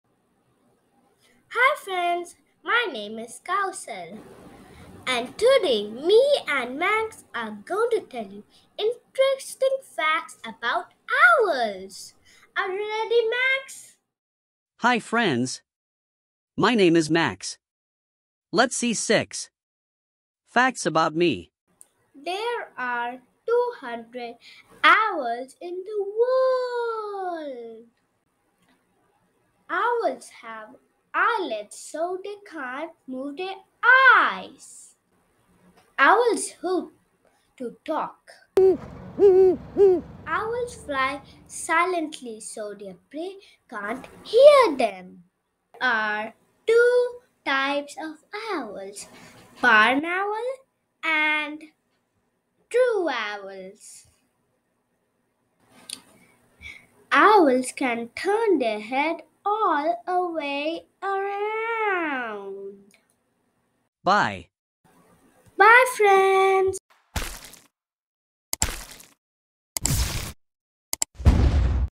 Fun facts about the Owl.
Fun facts about owl , kaushal and his owl friend Max will tell you 6 amazing facts about owl.